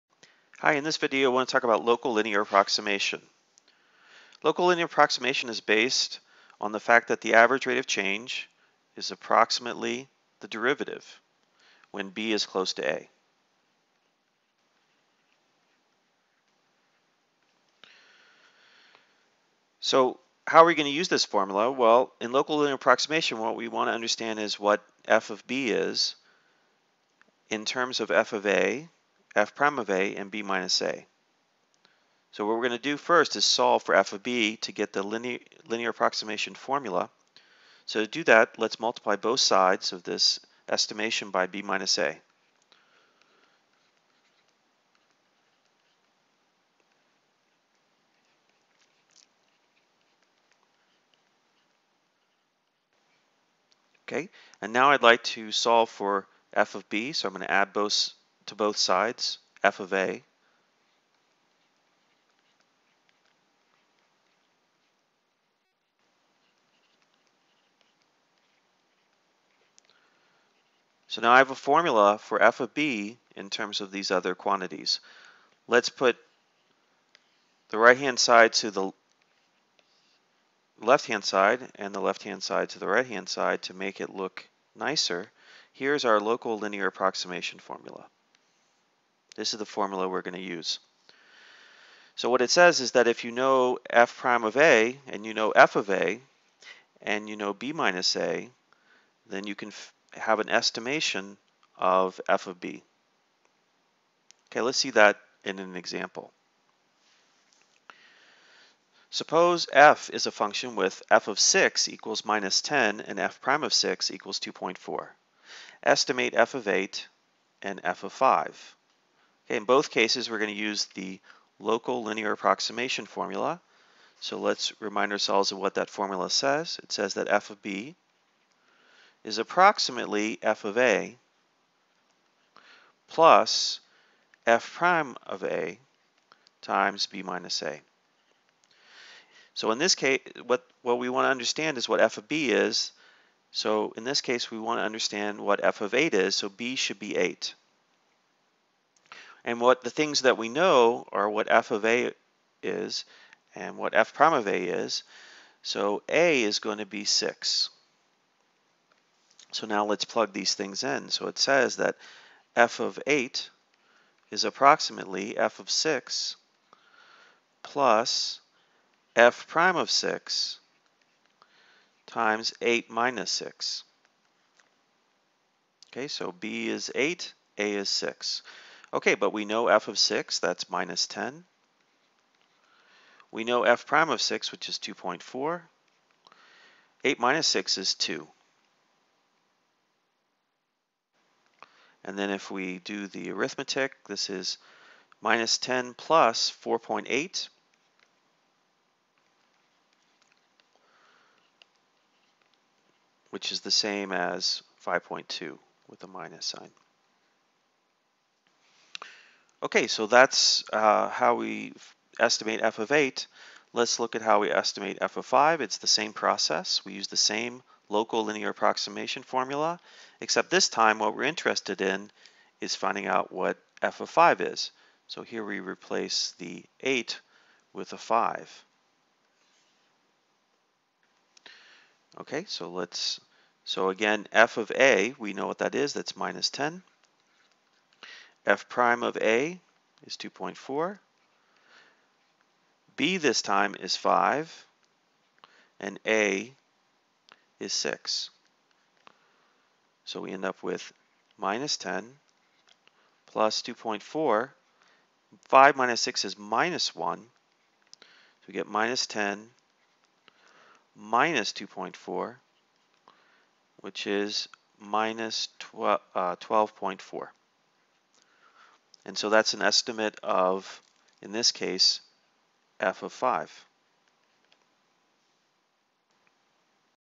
2.3.3 How to use local linear approximation
If you know f'(a) and f(a), then you can use local linear approximation to estimate f(b) for b that are near a.